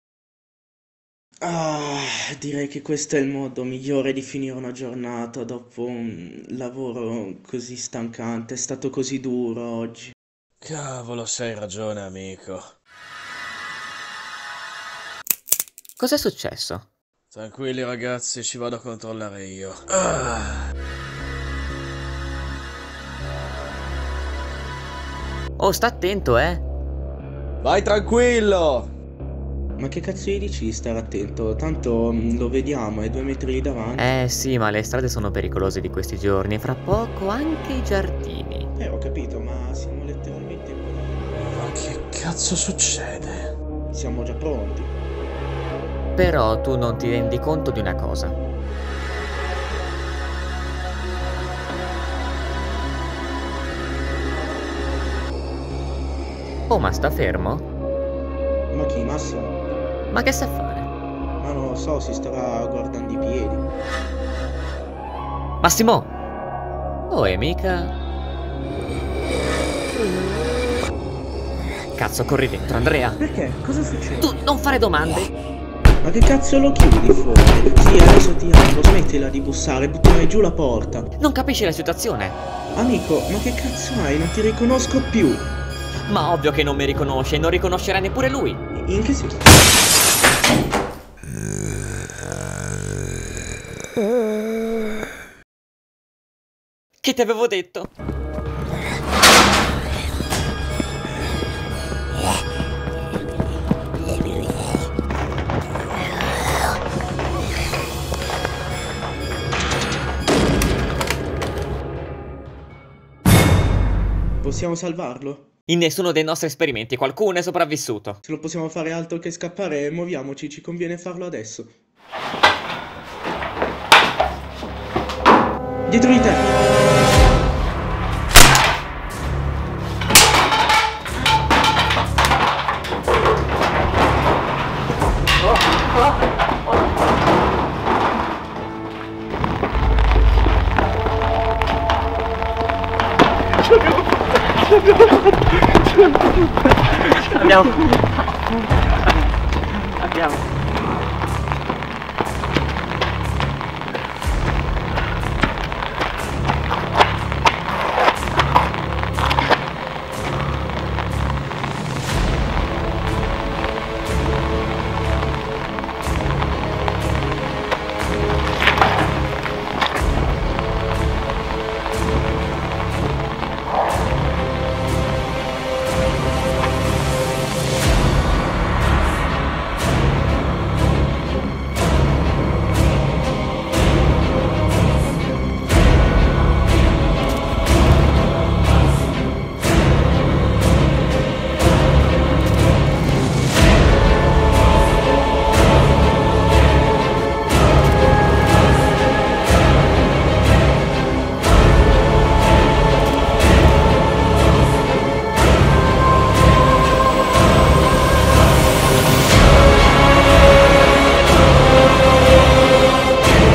Boletus ex imperium
the sequel to  the well renouned Boletus, the movie that took the world by storm, with a letal spore gas that  makes you a zombie ,like this video the likes keep me from falling in depression.
It's in italian i'll write a traduction

the people in the video in order to the credits